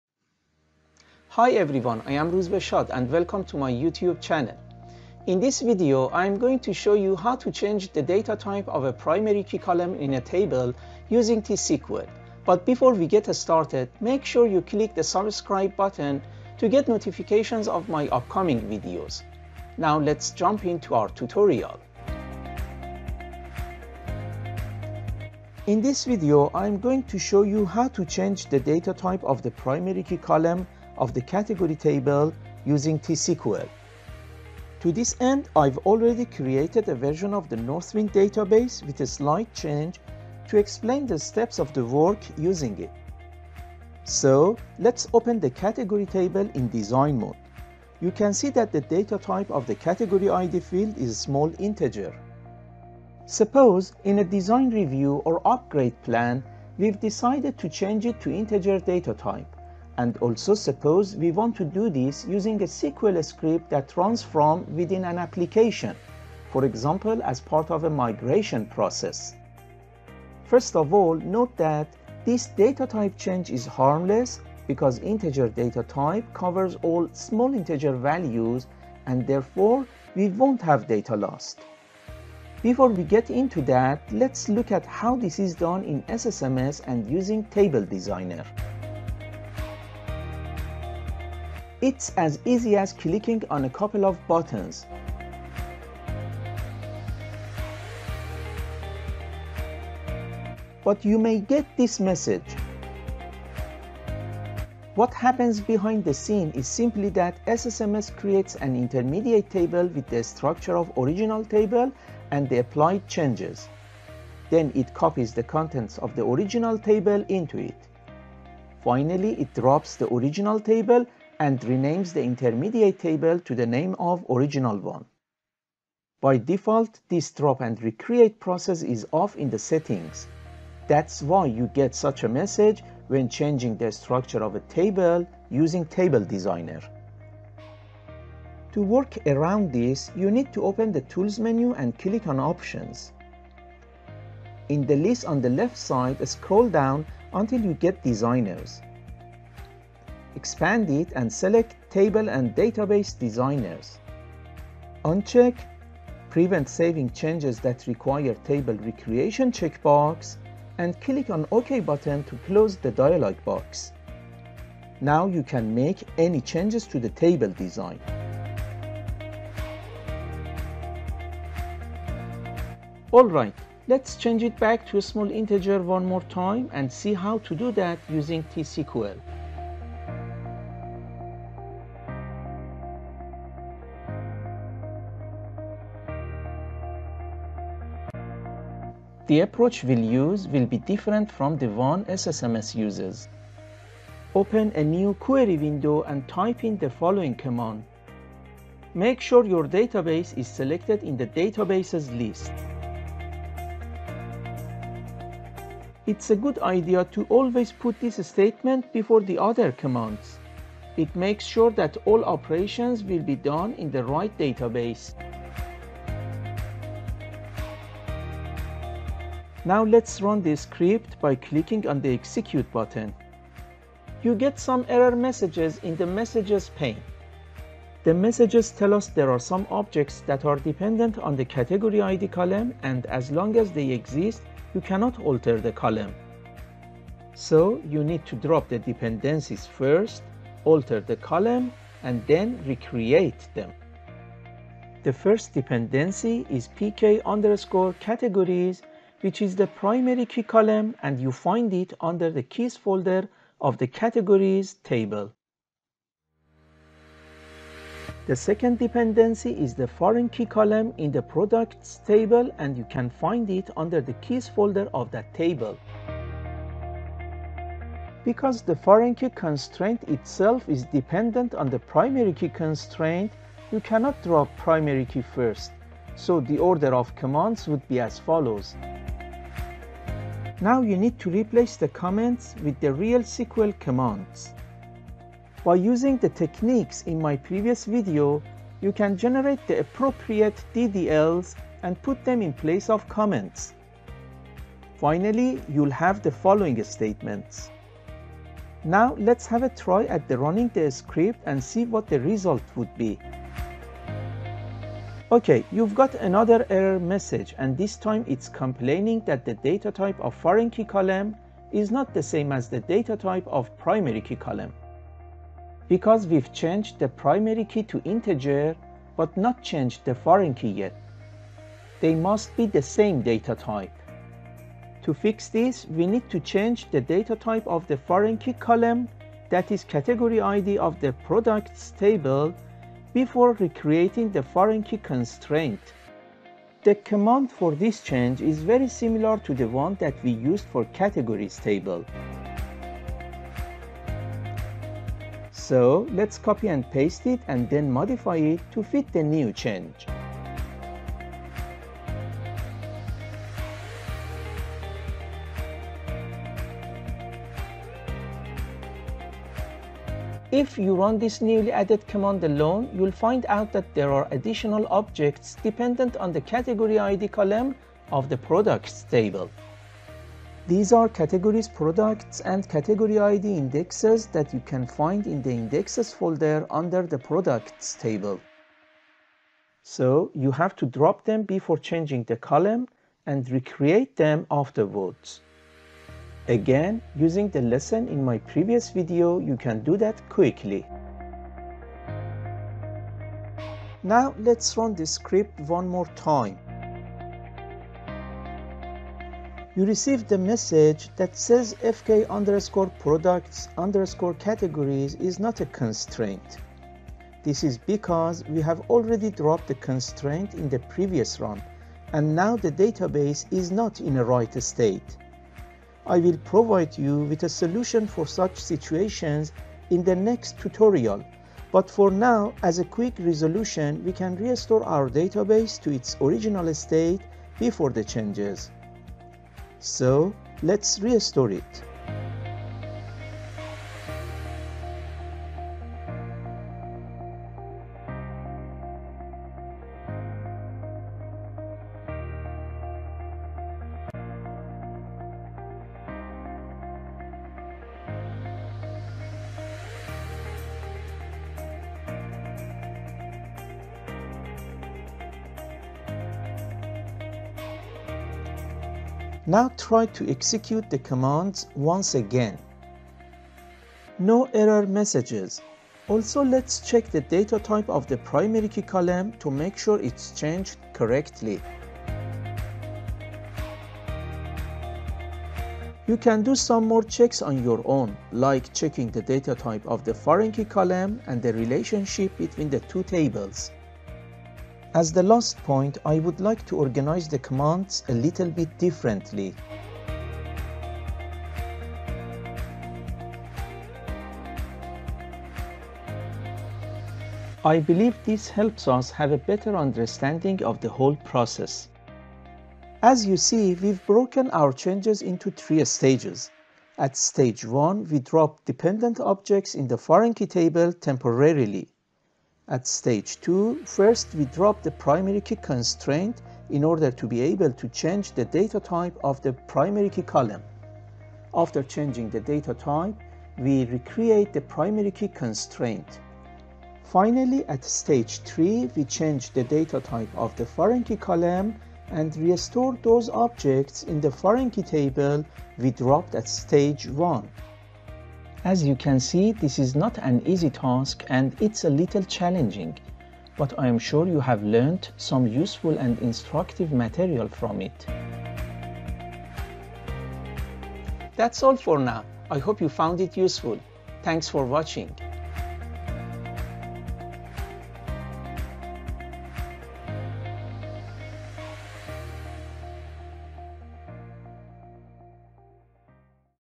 How to change the data type of PK column in a table using T-SQL
In this tutorial, I will show you the steps that are required for changing the data type of a PK column of a table that is in relationship with another table, by using Table Designer and T-SQL.